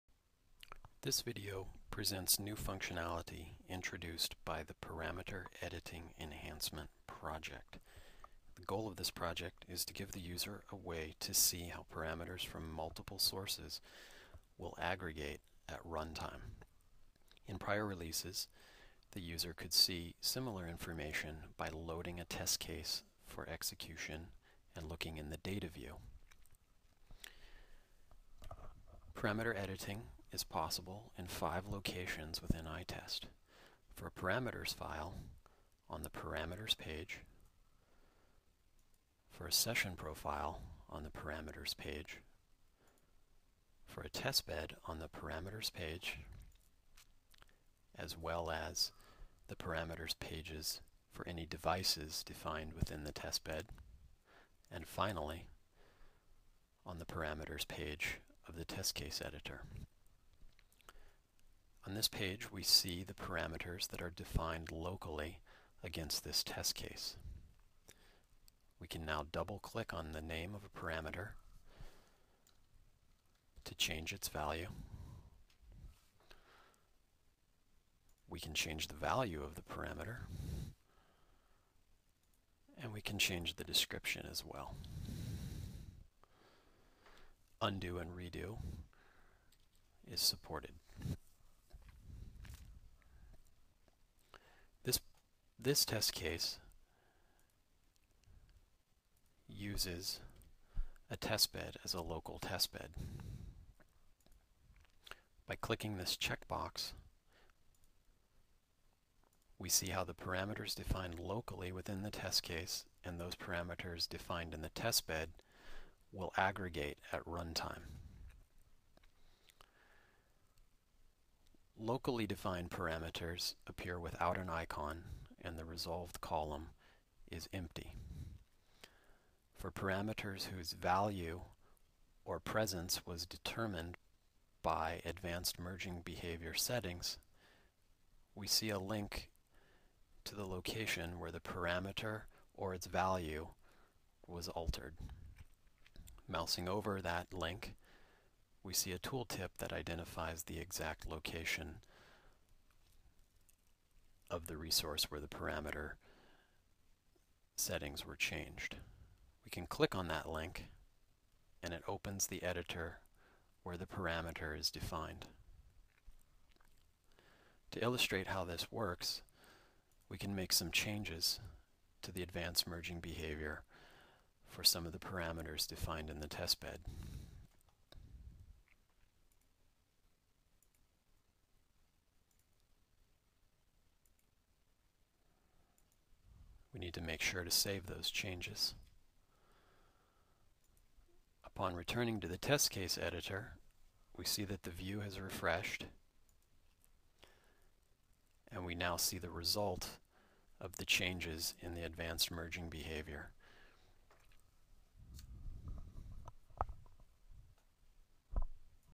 Managing test case parameter settings in Spirent iTest
Quickly identify merged parameters and view the values that will be used for execution. Double click to change parameter values and descriptions in grid. Click to reveal source of parameters in test cases.